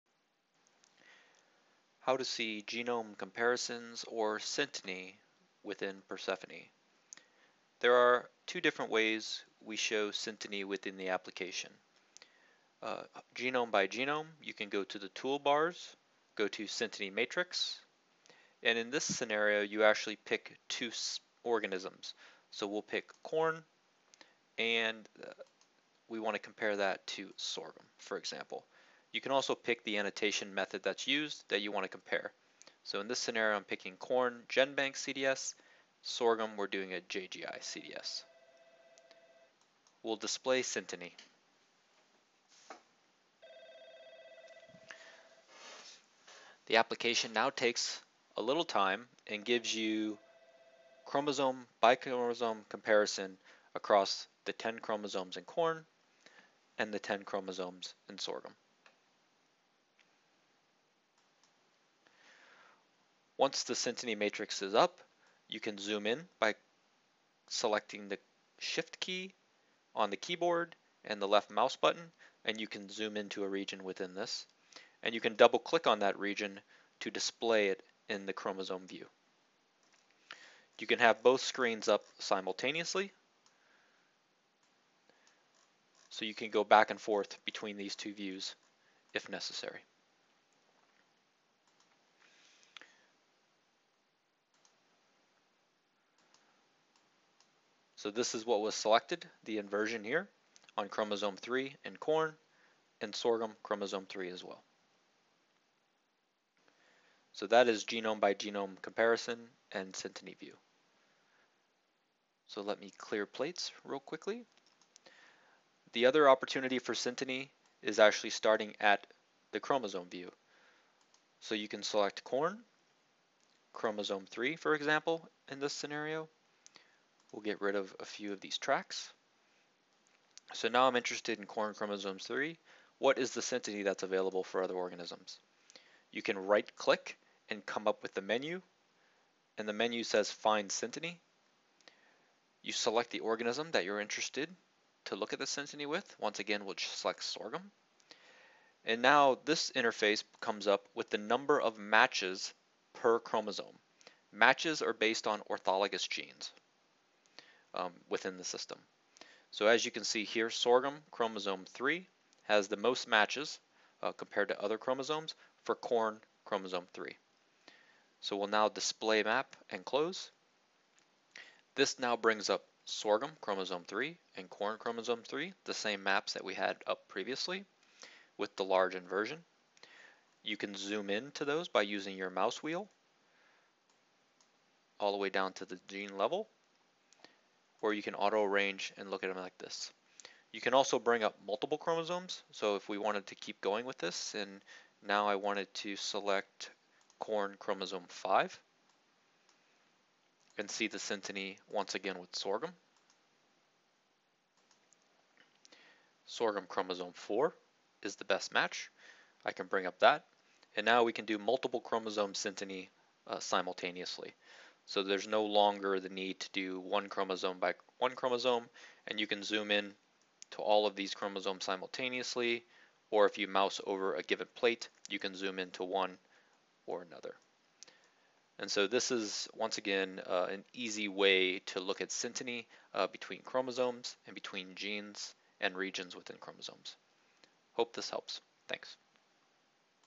Persephone - How to view genome comparisons (synteny)
a short video describing how to view and use genome comparison capabilities in Persephone.
to try it.